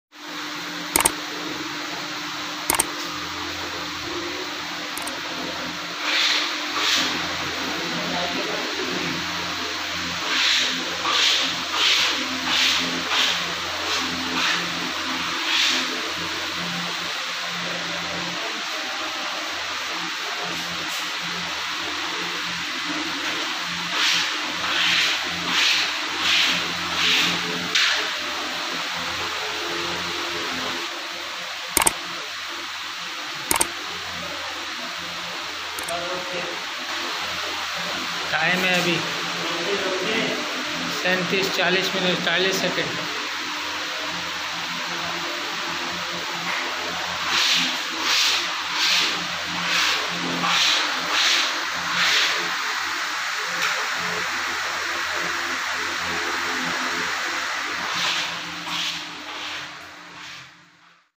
Beautiful Wallpaper in Very cheap price | New Design
Hello friends welcome to my Channel home design.To days short video we give you some design of wallpaper.wallpaper is the the perfect option for renters and homeowners who want a temporary solution to bare walls. In this video you will also watch how we renovate ower home and rooms.our video is about Wallpaper Design and pasting ideas
sabir wallpaper